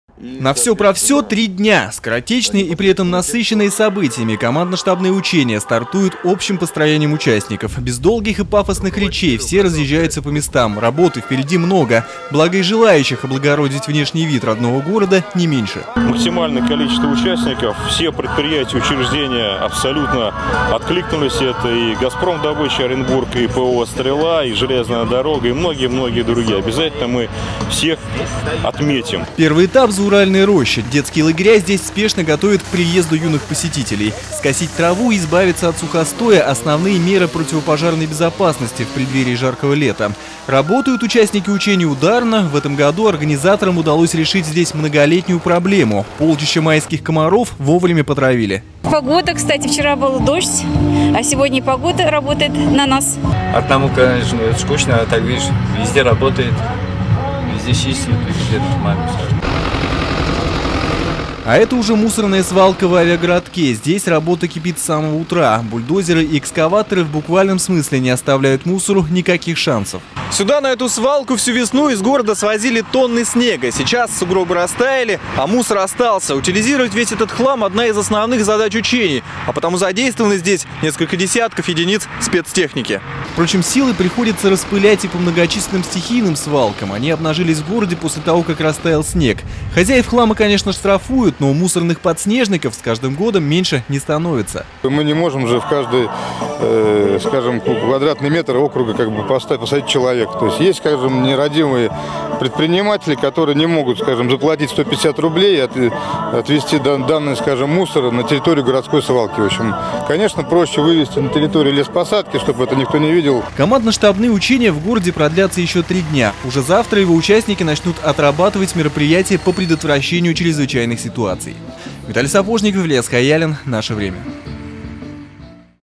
Генеральная уборка 22-05-13
Более двух тысяч человек и триста единиц спецтехники. Оренбург на три дня стал местом масштабных учений. Сегодня они начались с генеральной уборки в зоне отдыха «Дубки». В программе также ликвидация разлива нефти и предотвращение чрезвычайных ситуаций. Репортаж Виталия Сапожникова.